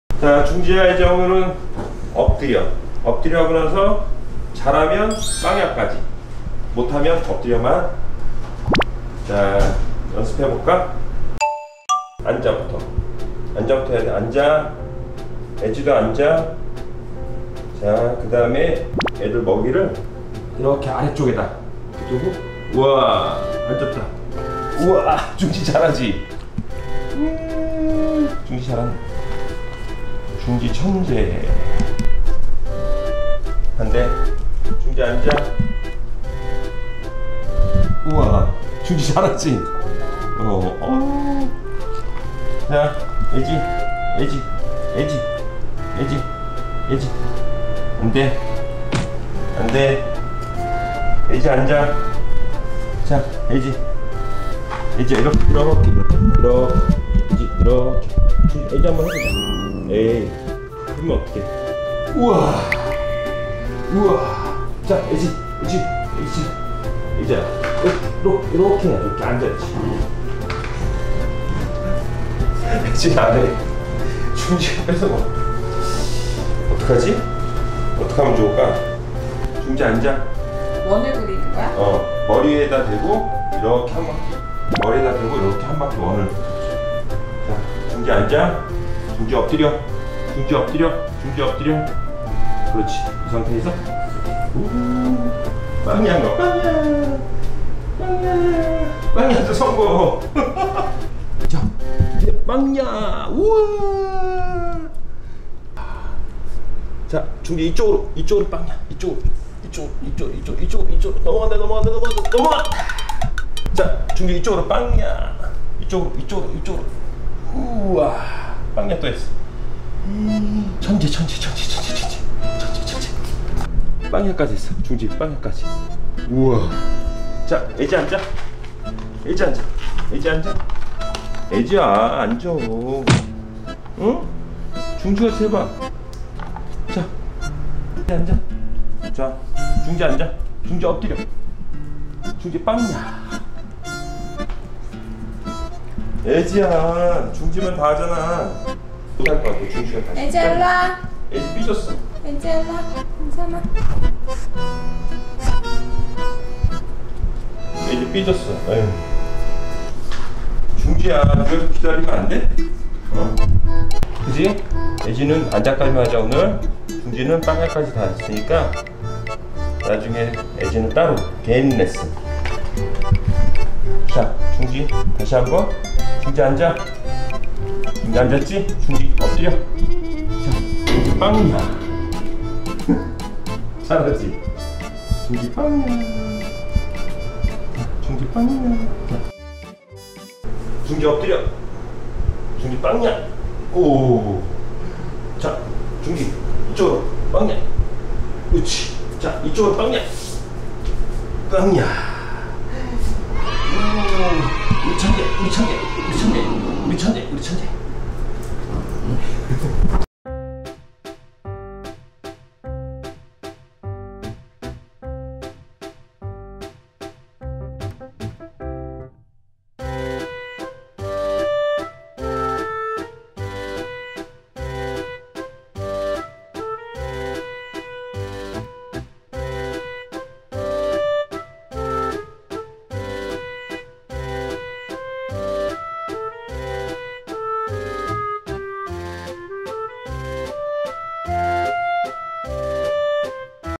[장마철 집에서 놀기 3탄] 비숑 강아지 5분만에 엎드려 빵야 훈련🐾How to teach puppies tricks in 5 mins🐾
애지중지랑 오늘은 엎드려! 빵야! 해봤어요 :)
정말 5분 만에 습득해버린 중지..! 반면에 애지는..?